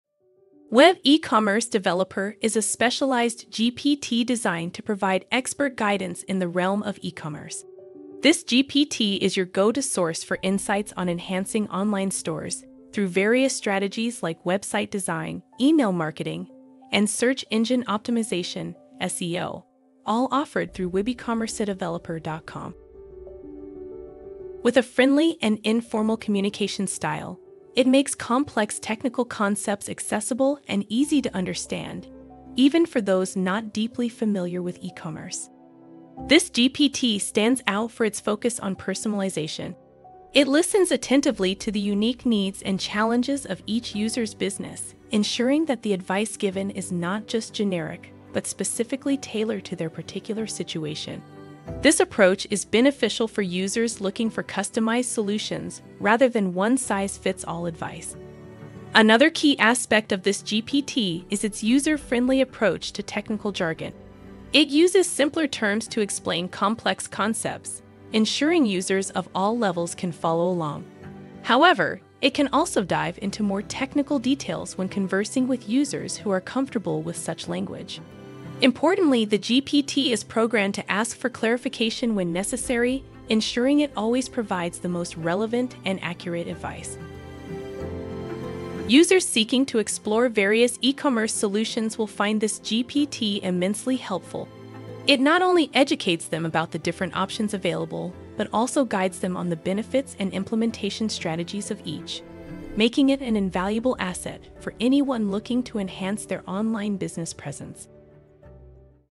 eCommerce GPT To Build Your eCommerce Brand WebeCommerceDeveloper.com
eCommerce GPT To Build Your eCommerce Brand

Web Ecommerce Developer is a specialized GPT designed to provide expert guidance in the realm of ecommerce. This GPT is your go-to source for insights on enhancing online stores through various strategies like website design, email marketing, and Search Engine Optimization (SEO), all offered through webecommercedeveloper.com. With a friendly and informal communication style, it makes complex technical concepts accessible and easy to understand, even for those not deeply familiar with ecommerce.